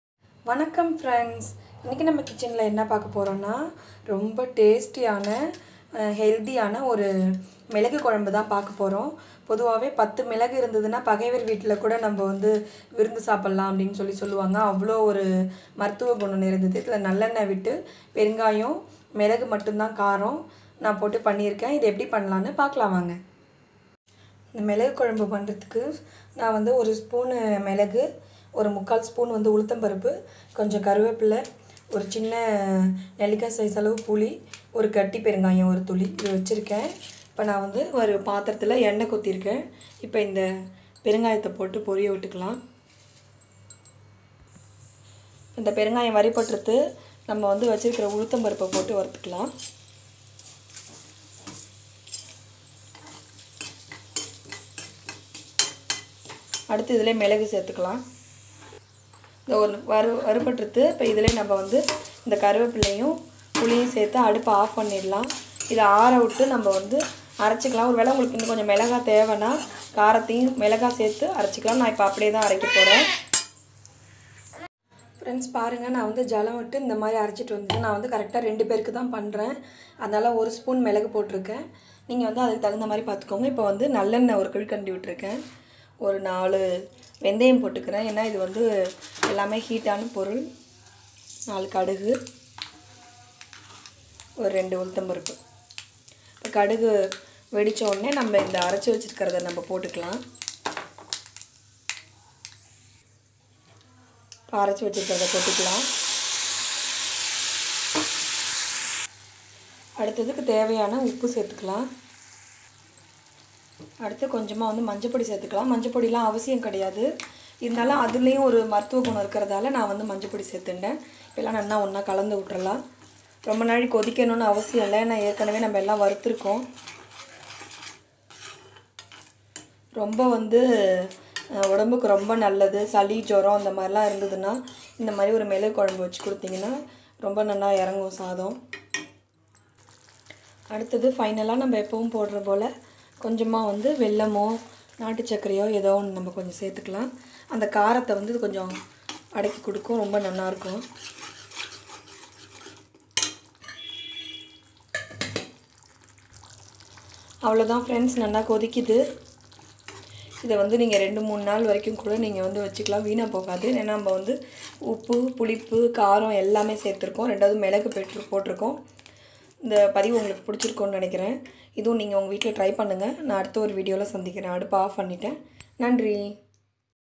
மிளகு குழம்பு - milagu kulambu in tamil
Tasty milagu kulambu

Milagu 1 spoon

Urad Dhall 1 spoon

Turmand

Turmeric

Gingaly oil

Asafofdiya